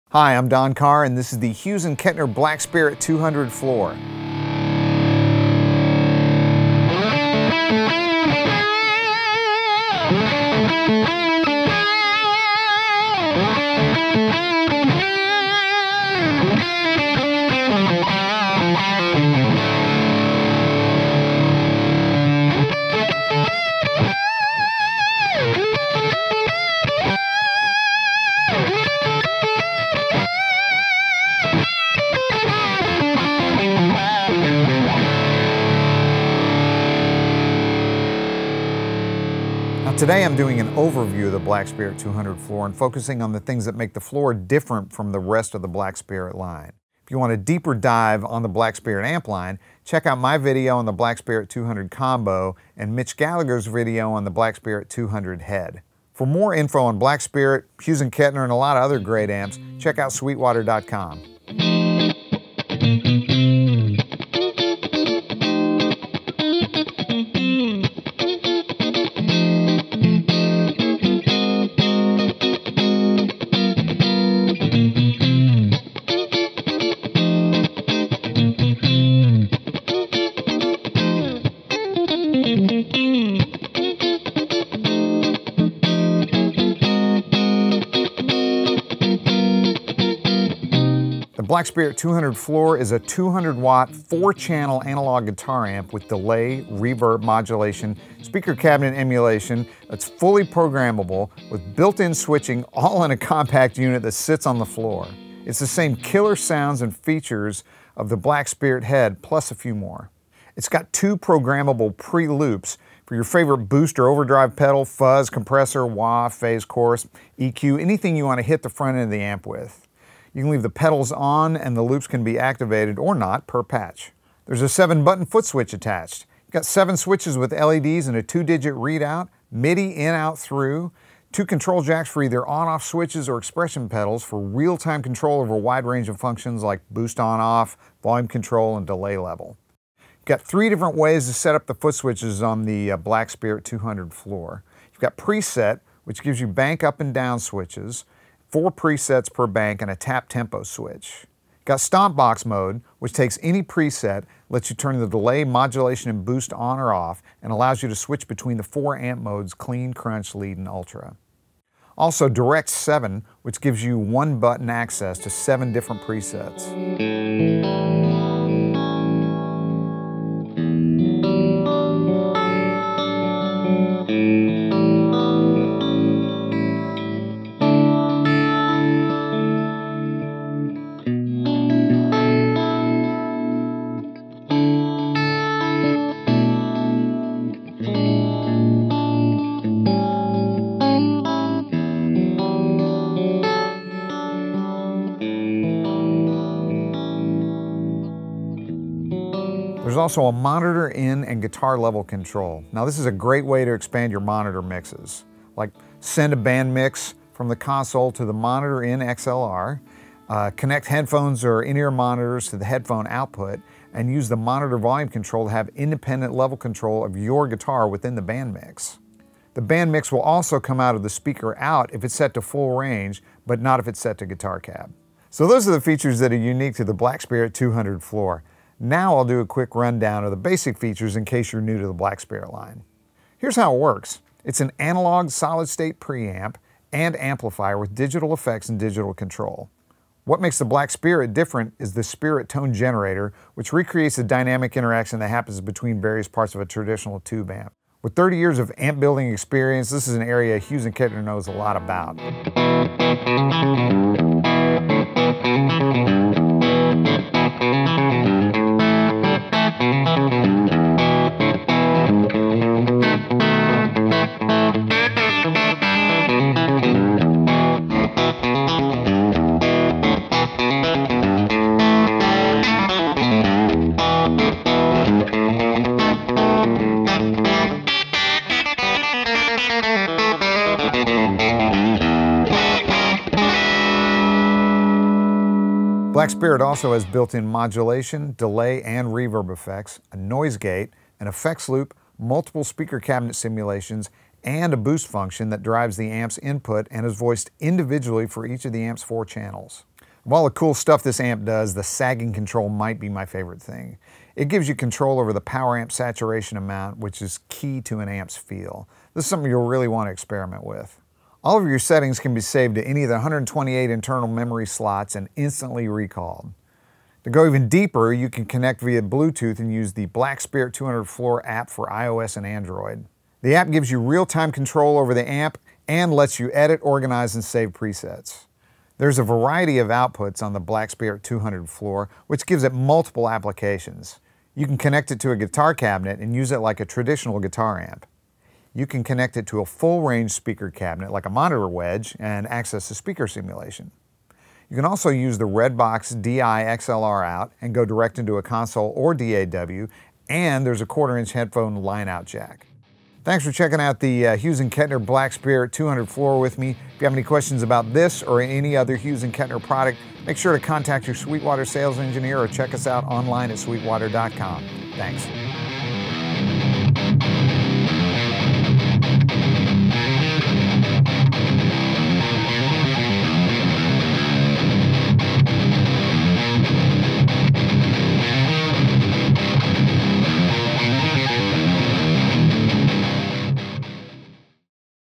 Hughes & Kettner Black Spirit 200 Floorboard Amp Demo
Shop for the Hughes & Kettner Black Spirit 200 floorboard amplifier at Sweetwater

Combining the sonic versatility of the Bionic Spirit tone generator with the practical functionality of the FSM-432 MIDI footswitch, the Hughes & Kettner Black Spirit 200 floorboard amplifier could be the new foundation of your guitar rig that you've been searching for. The Black Spirit 200 floorboard packs all the goods found in the Black Spirit head and combo amps, including 200 watts of power and integrated Red Box AE+ speaker emulation. You'll also find two additional true bypass pre-loops, and a dedicated monitor input for convenience.

After you watch, check out Sweetwater.com today for all your music instrument and pro audio needs!